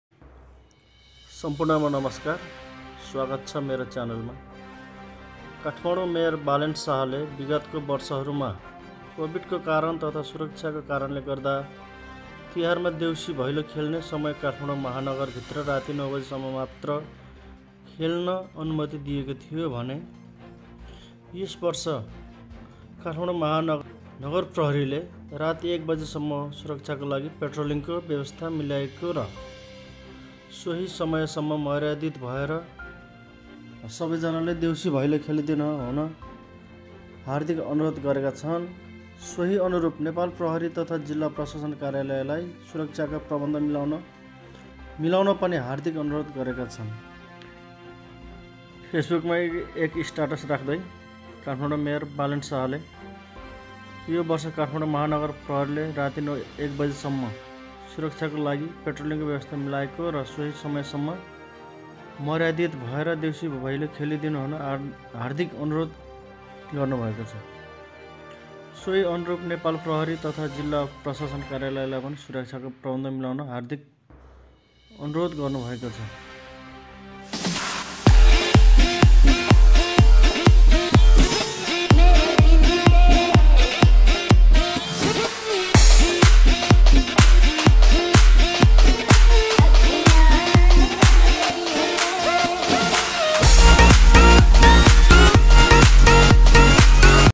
Mayor Balen Shah ले Kathmandu मा राती 1बजे सम्म मात्र देउसी भैलो काे लागी खोल्ने
Mayor Balen Shah ले Kathmandu मा राती 1बजे सम्म मात्र देउसी भैलो काे लागी खोल्ने | Balen Shah | JeetRanamagar